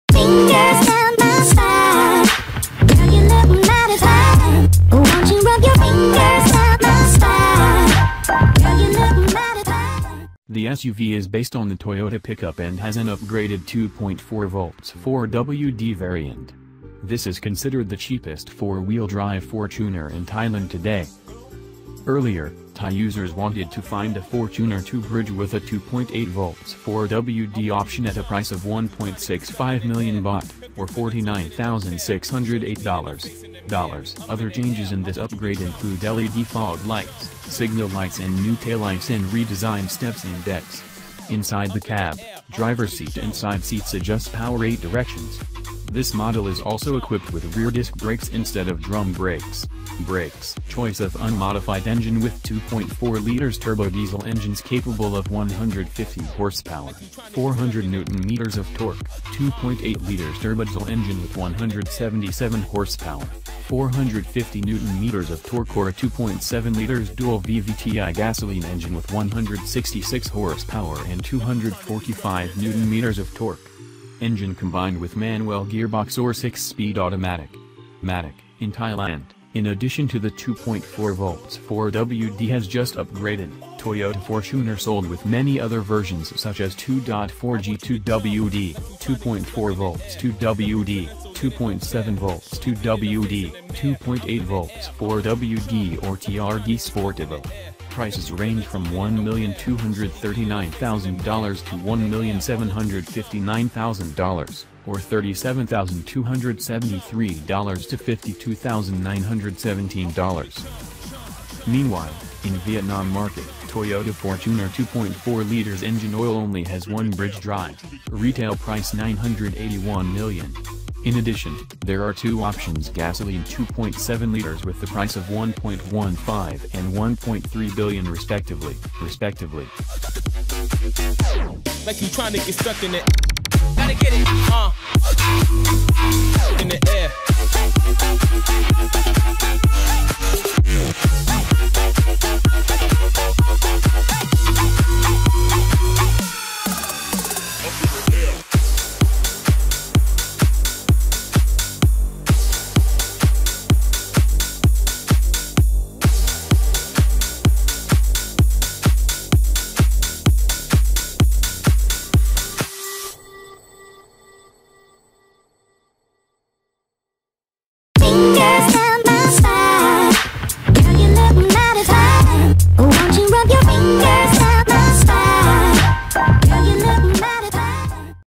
Toyota Fortuner 2.4V 4WD is priced at $ 45,000 in Thailand | Car and girl
Toyota Fortuner 2.4V 4WD is priced at $ 45,000 in Thailand
The cheapest Toyota Fortuner SUV in the Thai market has been sold for $ 1.5 million, or $ 45,000.